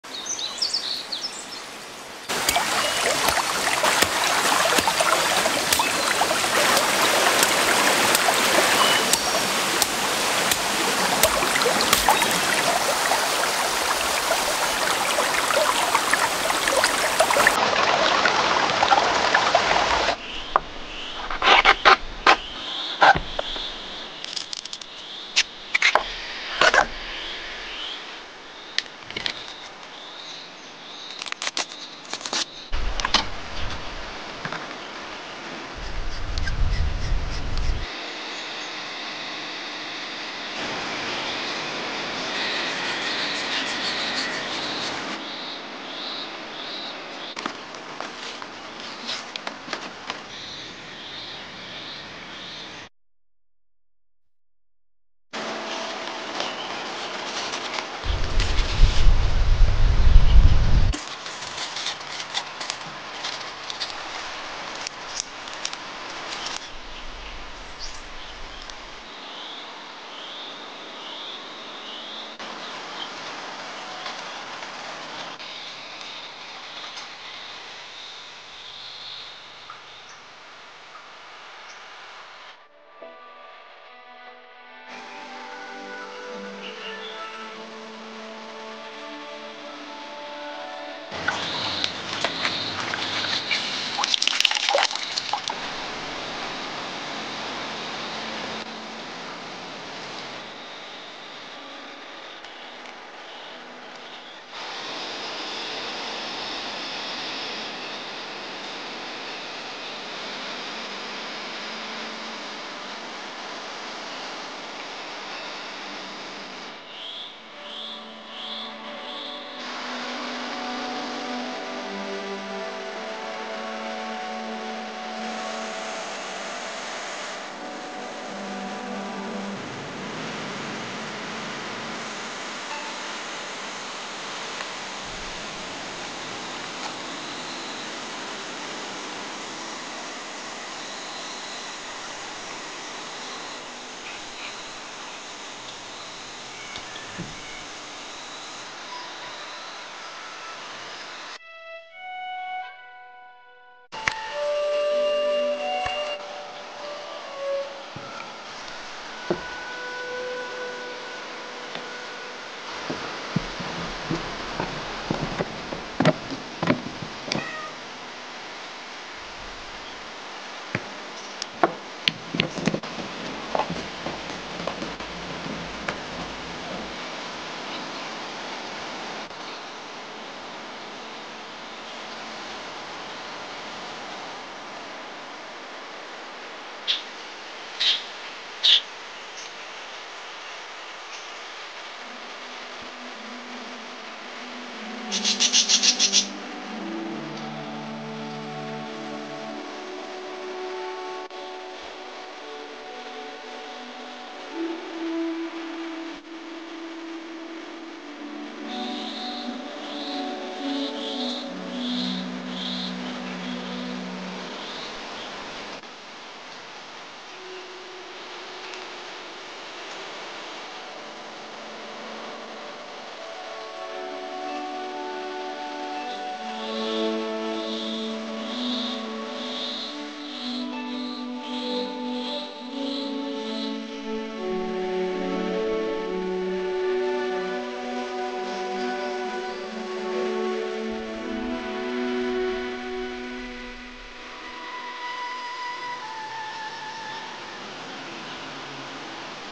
Reviving the Ancient Everlasting Flower - Chinese Peony with Wood, as Stunning as the Real Thing
Hello everyone,
Experience the charm of traditional culture from the perspective of the ancients!
It is an honor to meet you!